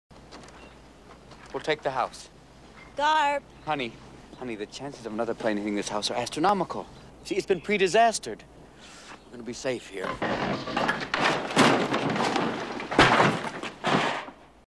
Gambler's Fallacy - World According to Gump example - Fallacious Trump e111
The Gambler's Fallacy is committed when someone confuses dependent and independent events or claims that a particular outcome is 'due' because it hasn't happened for some time.